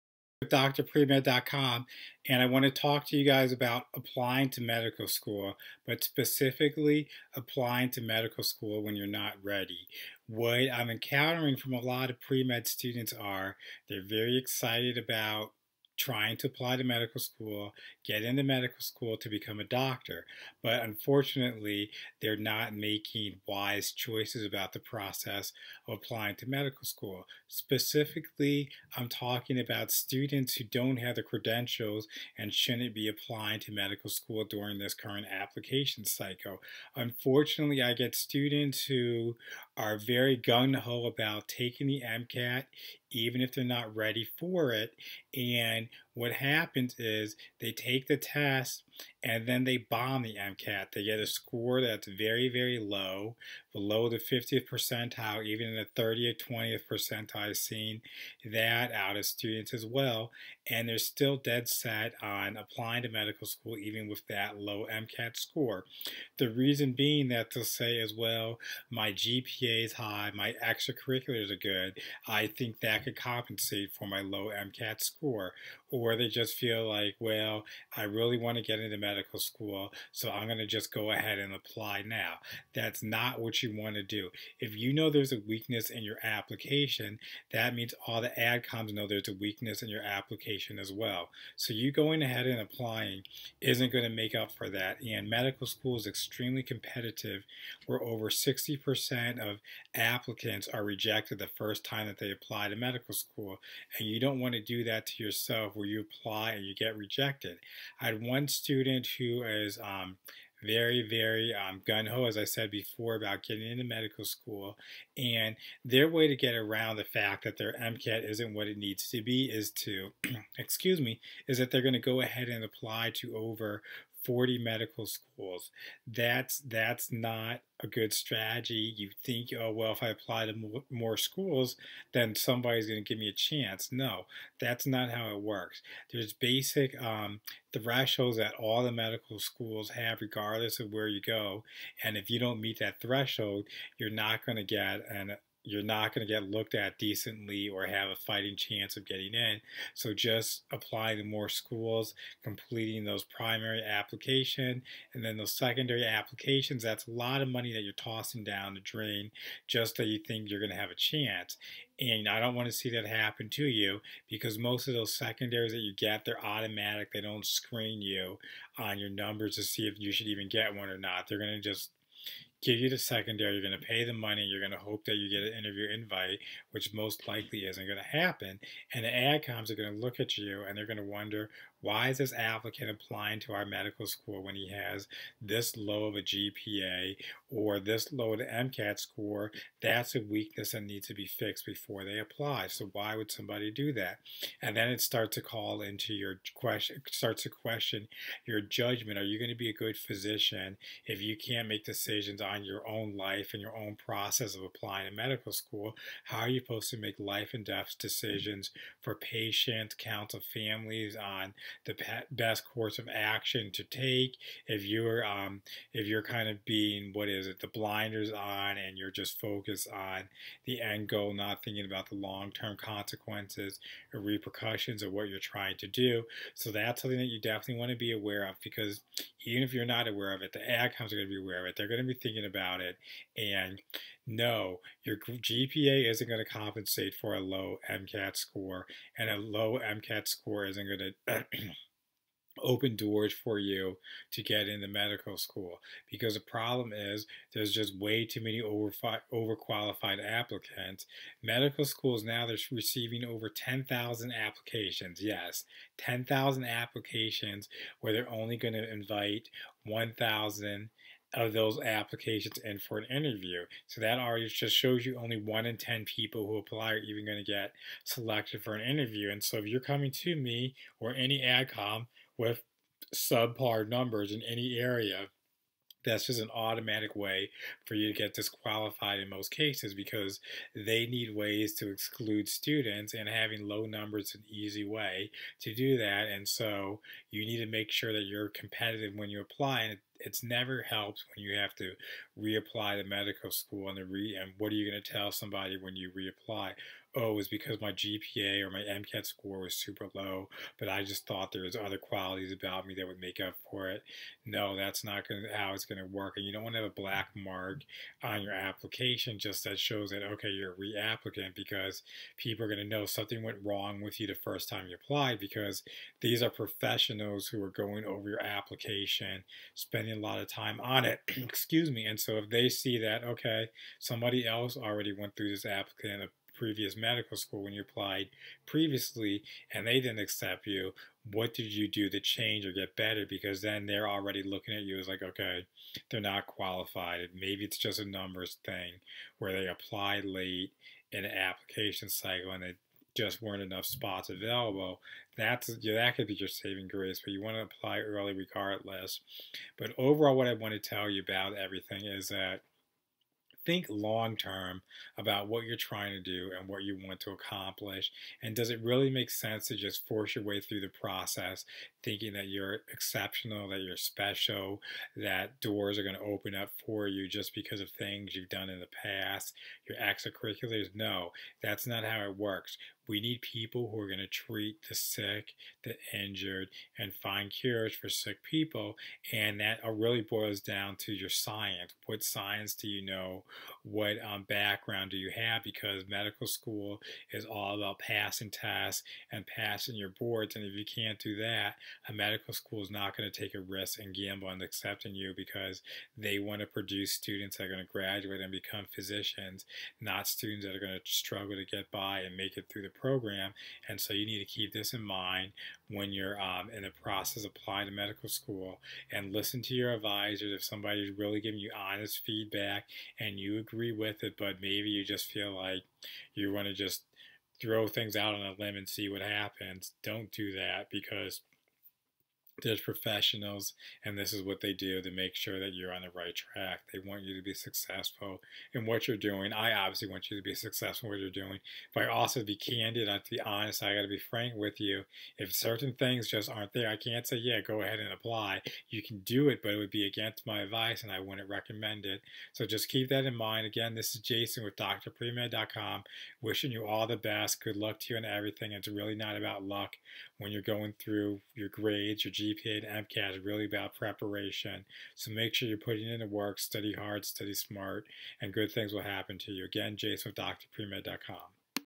Applying to Medical School Bad MCAT Score
You have a bad MCAT score but you still want to apply to medical school. Here's how adcoms will view your medical school application. Do your extracurriculars and GPA makeup for poor MCAT?